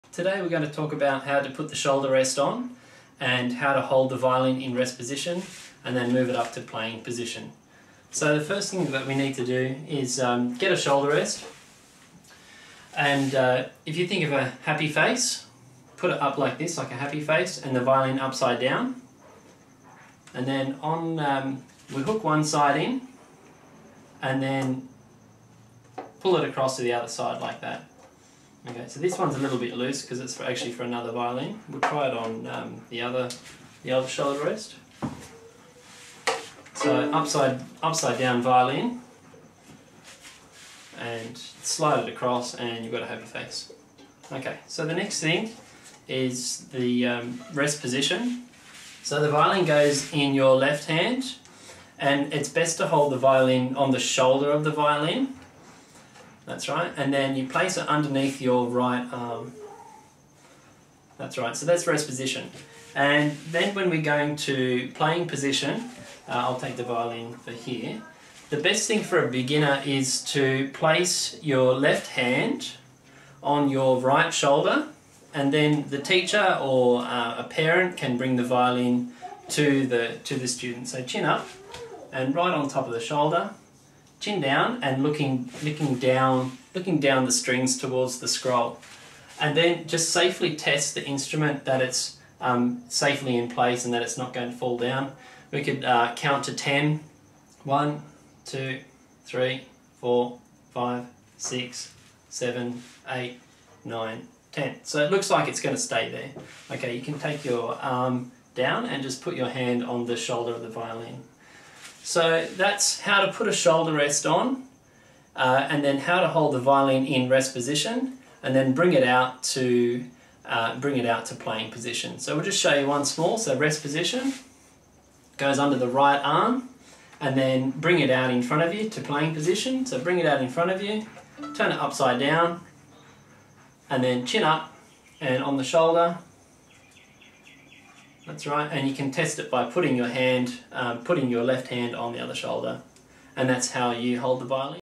How to put on a shoulder rest and hold a violin / viola
We show you how to put on a shoulder rest on.
How to hold a violin/viola in rest position and playing position.
for the Frog and Fish bow holder available at Sydney String Centre